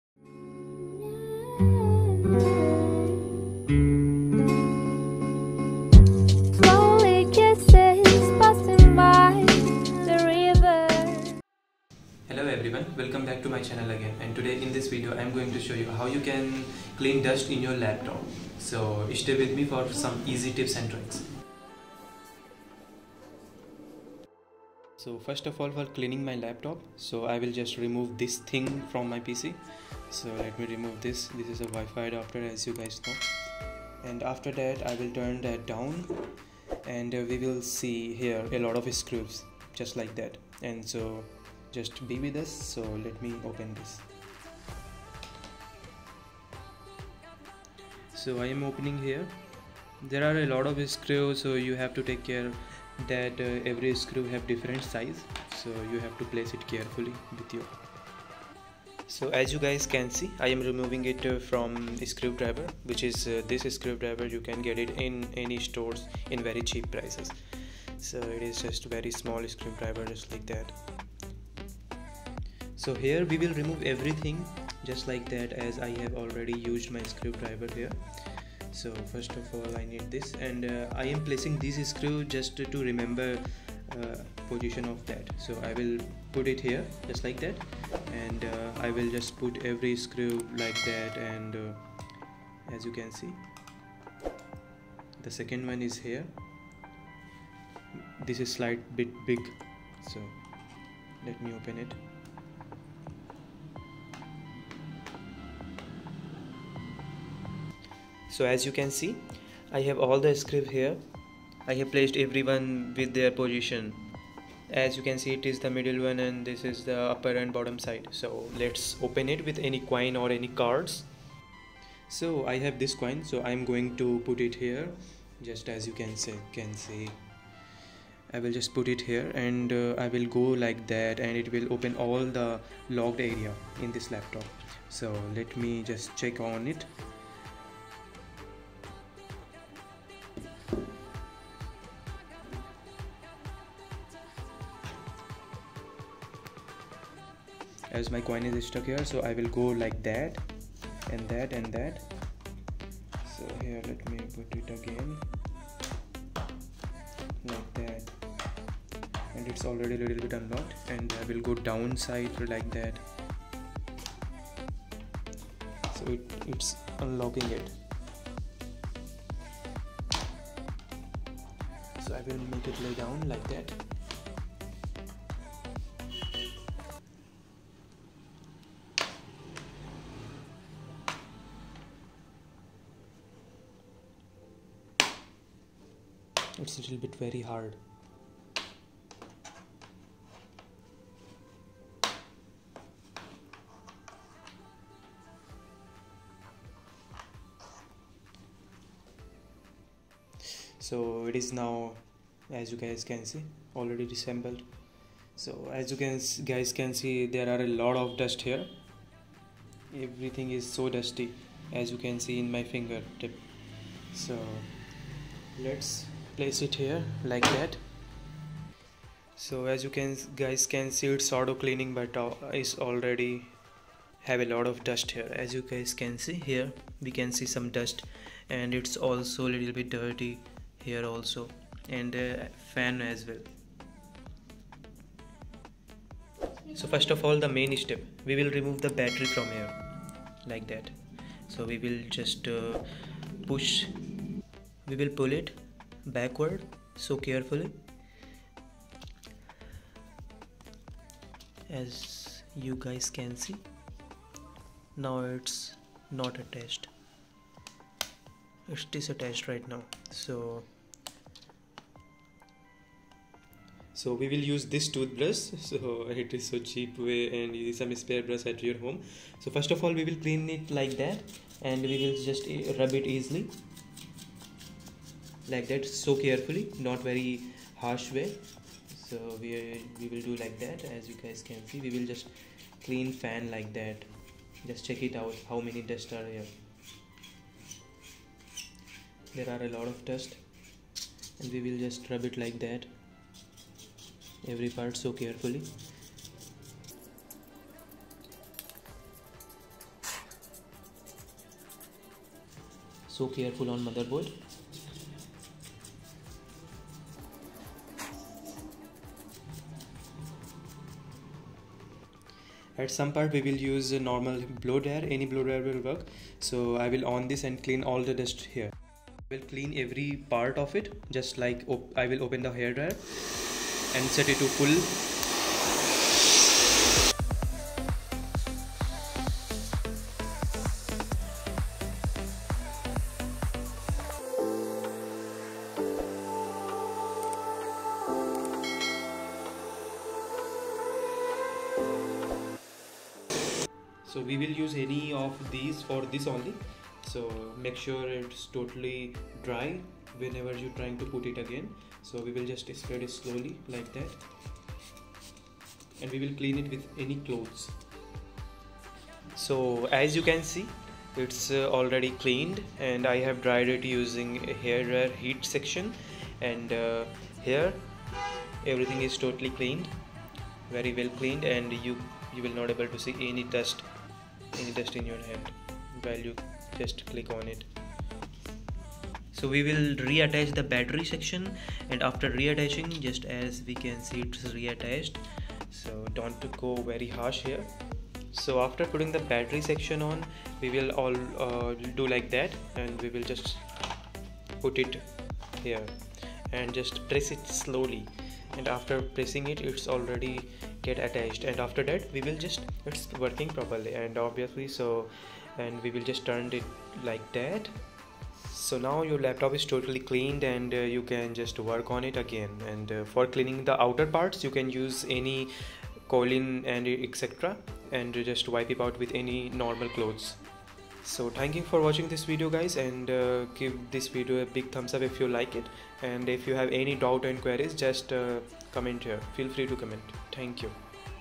How to Clean Dust from Asus Tuf A15 & F15 | 0 To 100%
If you are thinking to clean your Asus Tuf A15, Asus Tuf F15 or Similar to that model feel free to watch this video.
Here i have explained about How to Clean Dust from Asus tuf a15 laptops